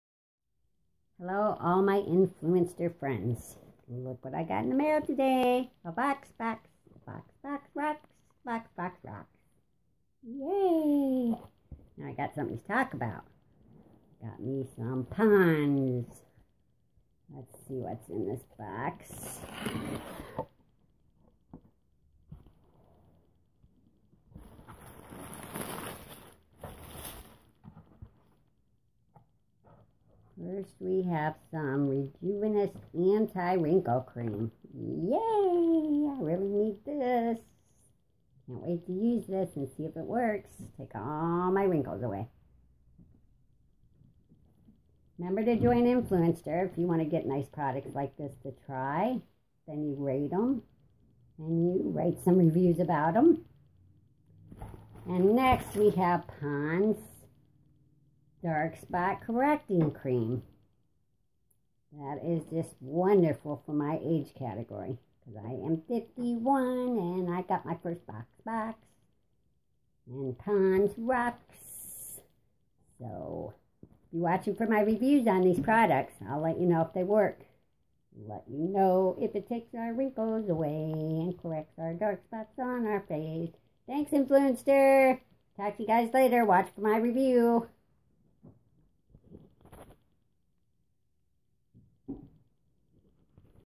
My very first Vox Box from Influenster!!!
I am so glad I joined Influenster. I get to try out products and then leave reviews. I will be trying out these two products from Pond's.